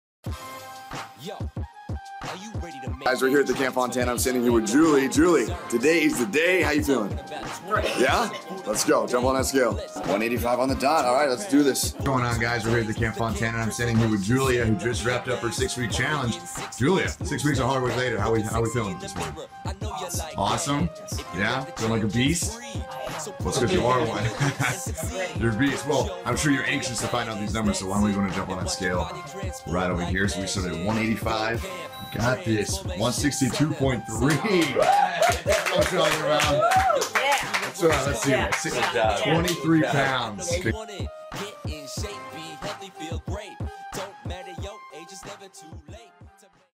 Fontana Fitness 6 Week Challenge Result - Julia Woodward
We will now check the result of Julia's 6 week weight loss challenge!